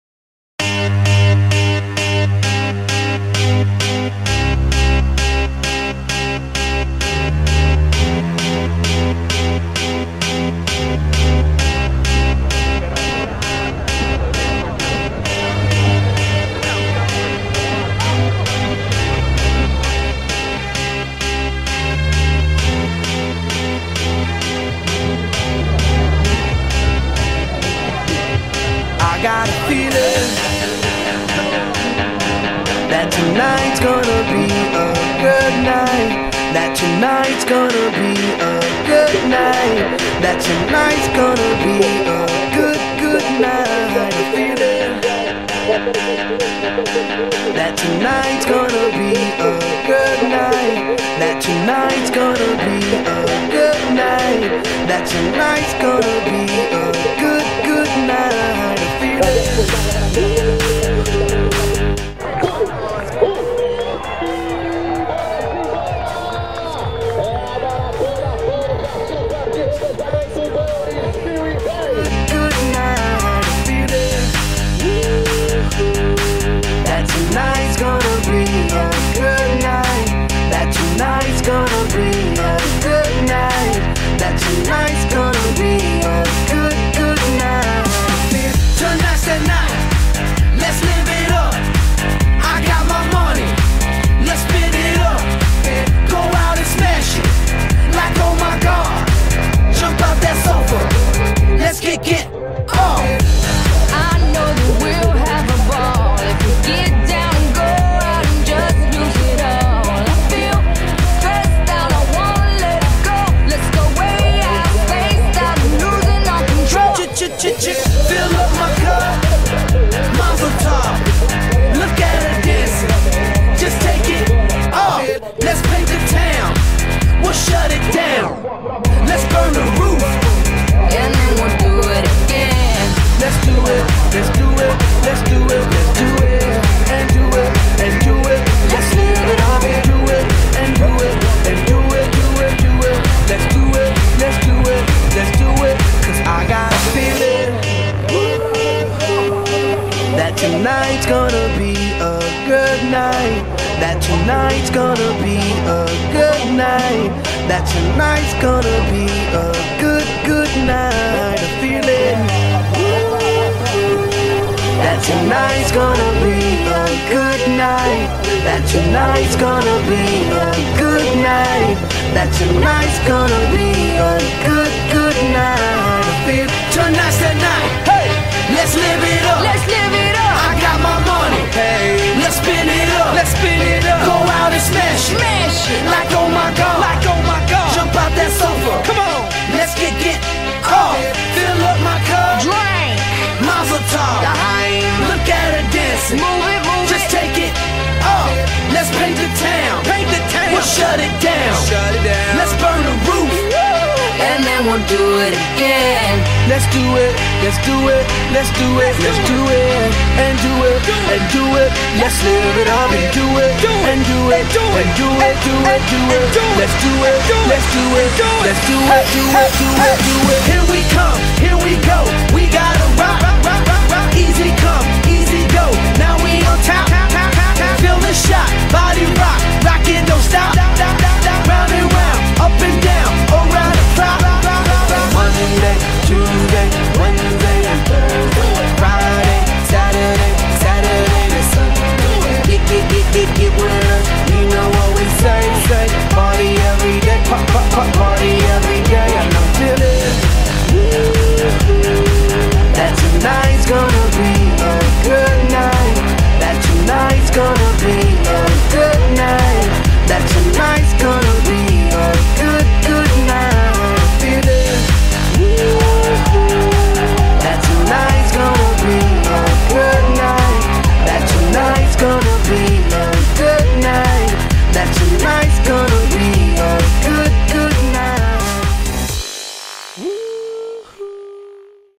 Maratona de Revezamento 2010 - Equipe Kobold
Participação da Equipe Kobold (Fernando Ribeiro, Regina Soares, Celio Torrencilha e André Pavini) na 18a. Maratona de Pão de Açúcar de Revezamento 2010.